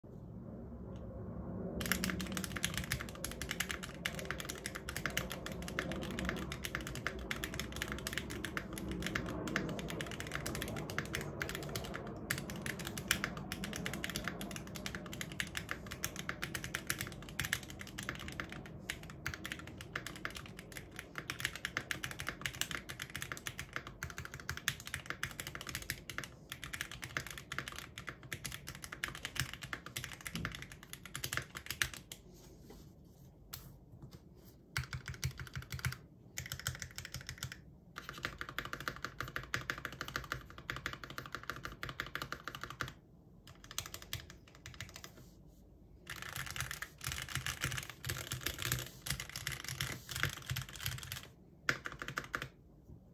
Tofu 65 Updated Typing Test
The keycaps have been changed for some cheap double shot pbt WOB ones.
Spacebar switch has been changed to a JWK matcha. (lubed)
Stabilisers have been (mostly) fixed with a dialectric grease syringe, really useful mod I learned off of alexotols.